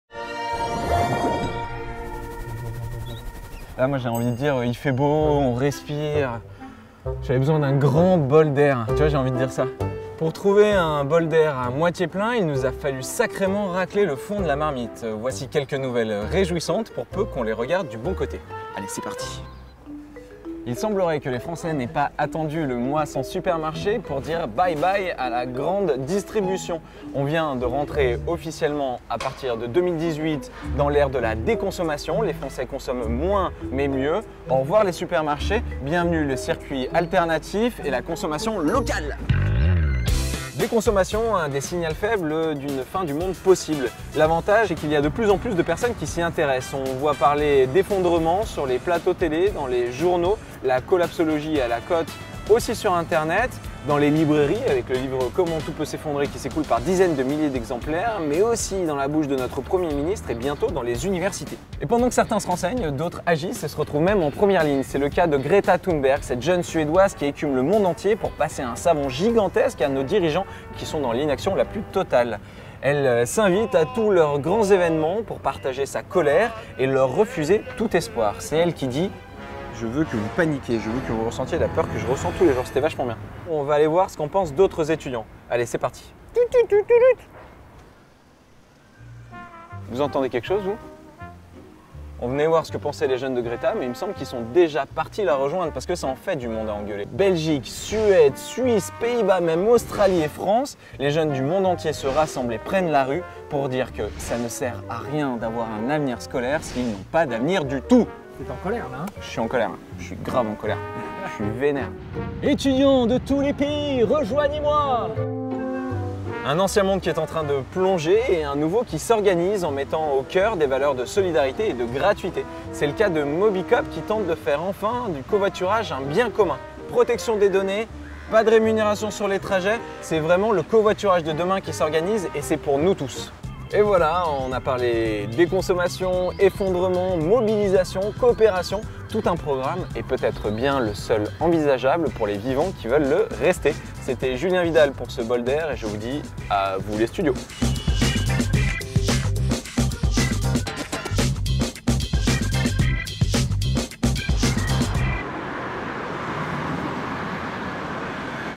Le Bol d'R | Fév. 2019
Voici les bonnes nouvelles du dernier mois écoulé, par Julien Vidal. Réalisé par Cyril Pallus.